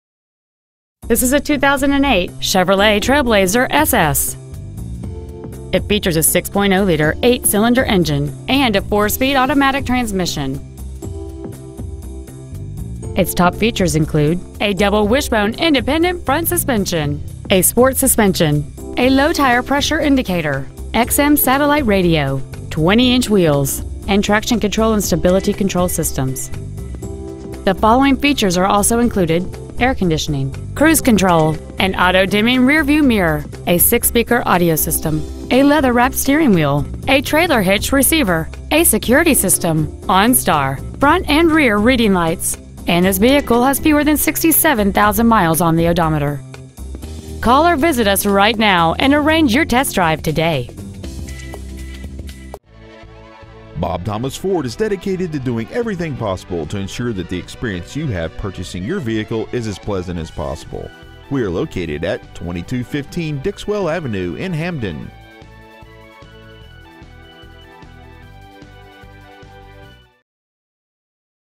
2008 Chevrolet Trailblazer SS Hamden CT
We are proud to present this 2008 Chevrolet Trailblazer SS .

 We've been honored to serve the Hamden CT area , we promise that your experience at our dealership will exceed your expectations !

 Year : 2008
 Make : Chevrolet
 Model : Trailblazer SS
 Engine : 6.0L V8 16V MPFI OHV
 Trans . : 4-SPEED AUTOMATIC
 Exterior : BLACK
 Miles : 66,982

 Stock : T2154A